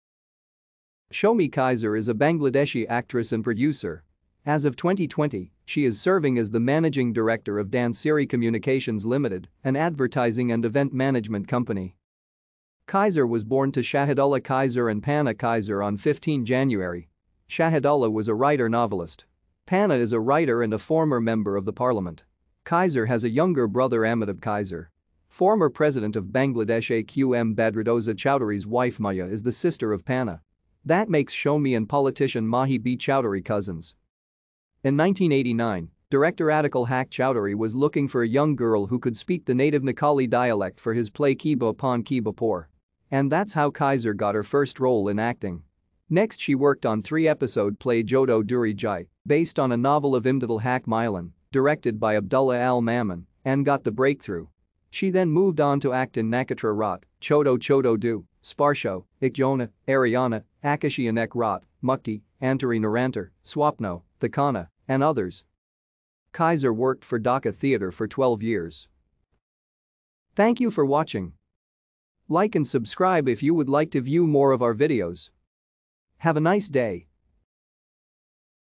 Shomi Kaiser
Shomi Kaiser  is a Bangladeshi actress and producer. As of 2020, she is serving as the managing director of Dhansiri Communications Ltd, an advertising and event management company.